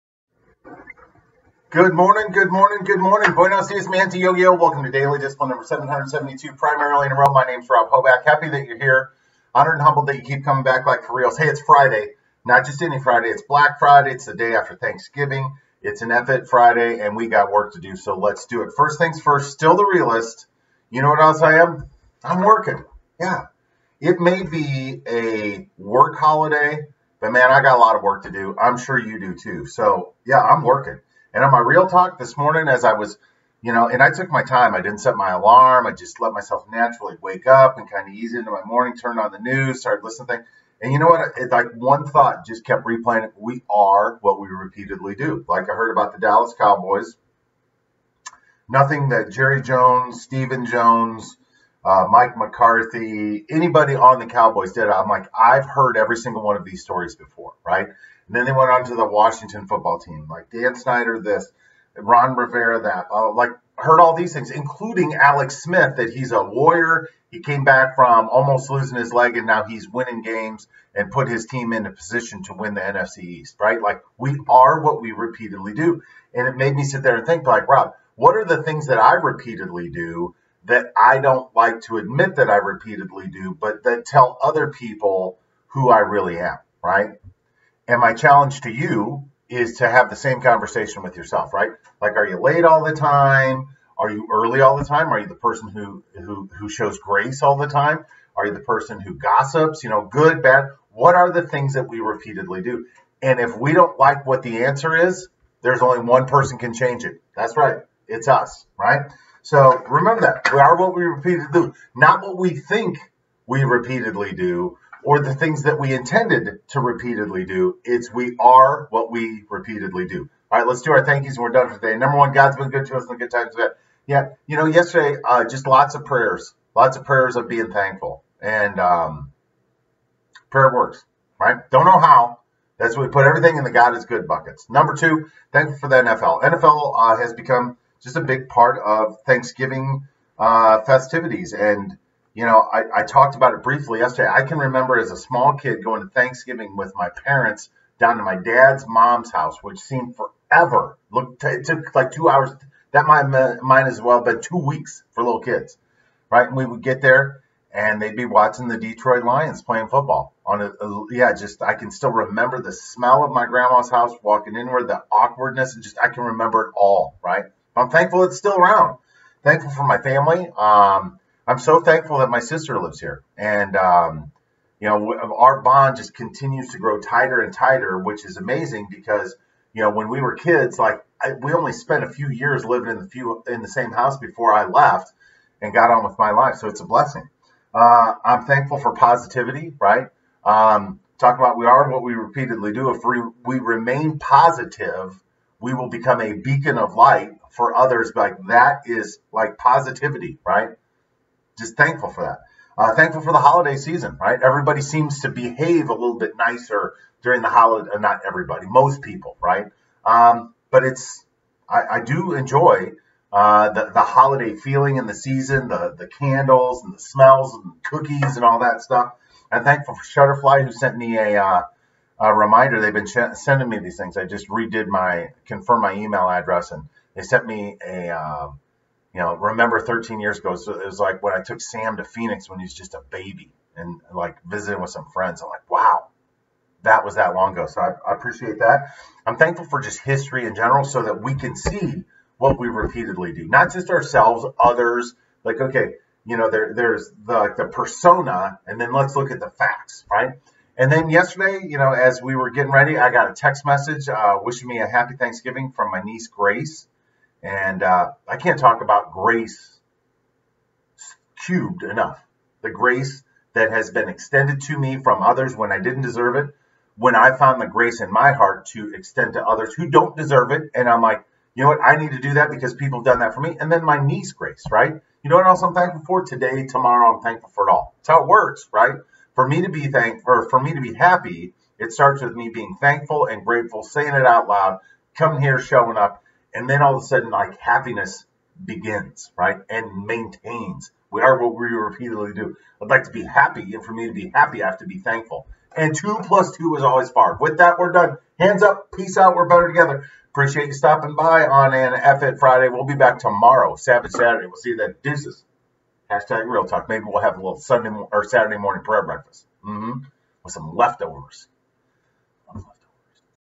DD #772 - We are what we repeatedly do
Hello and welcome to DD #772.  What is it that you repeatedly do that defines who you are?  Do you know?  Are you sure?  Does your vision of who you are match up with your results?  We are what we repeatedly do, now what we meant to do, or thought about doing.  We are what we repeatedly do.

Rob